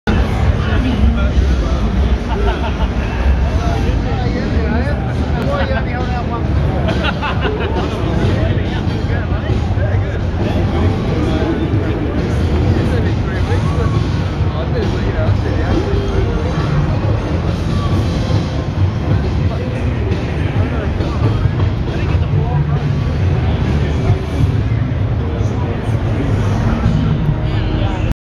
E3 2009 Raw Day 01: Batmobile
I took a bunch of video at E3 today with the intent of editing it together as I did in the past, but this digital age has made me lazy. So I'm calling it "Raw" to make it sound edgy, but it really just means "no effort".